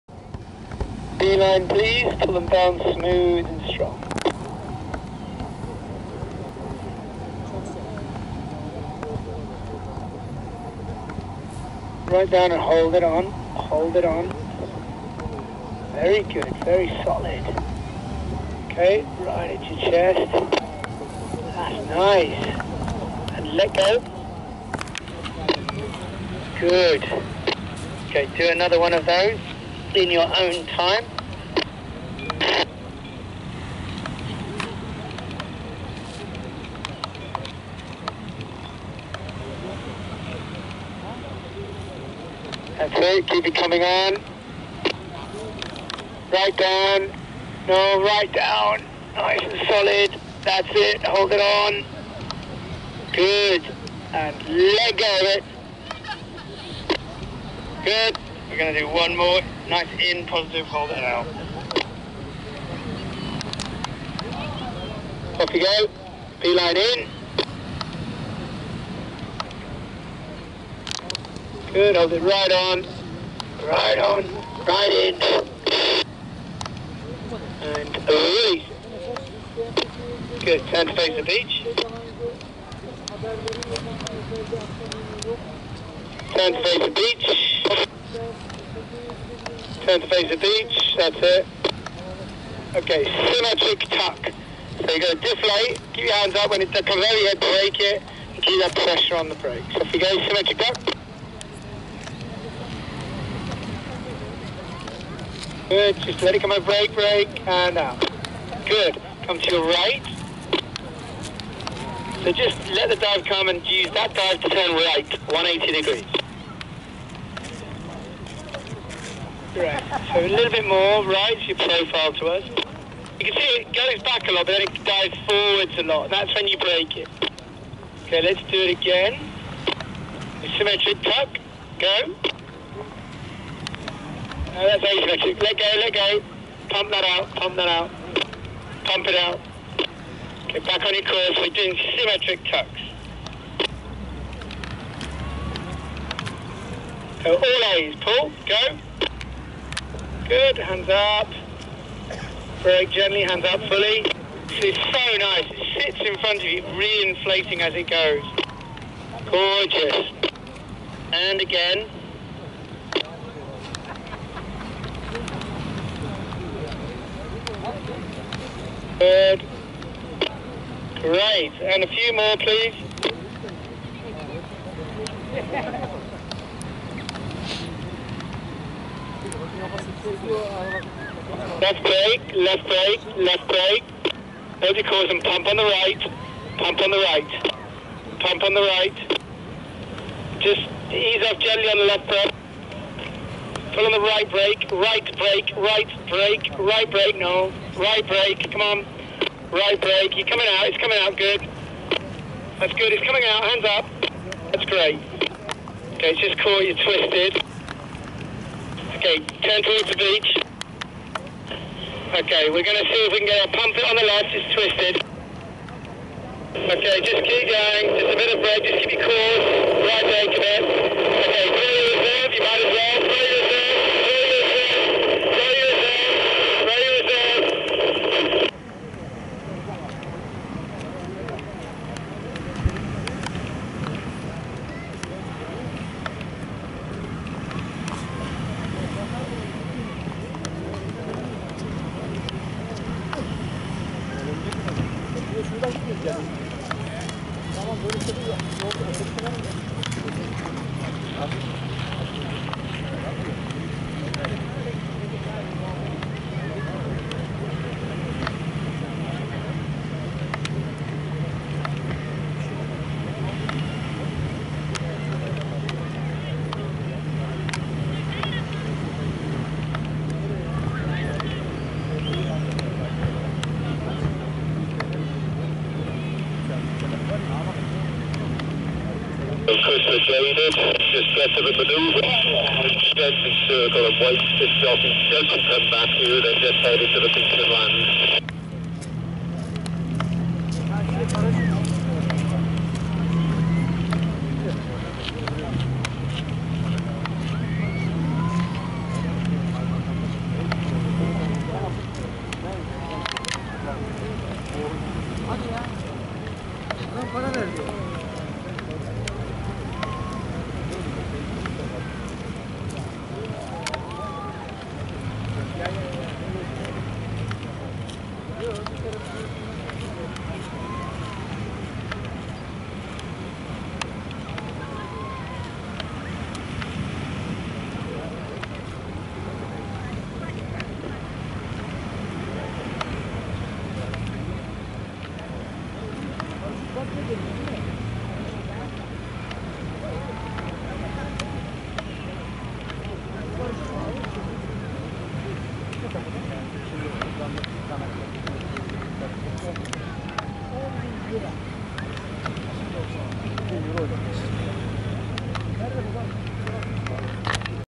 SIV - Turkey - 28-9-2017
SIV course in Turkey. B-line stalls and symmetrical tuck. Line over causes spiral and crash into the water.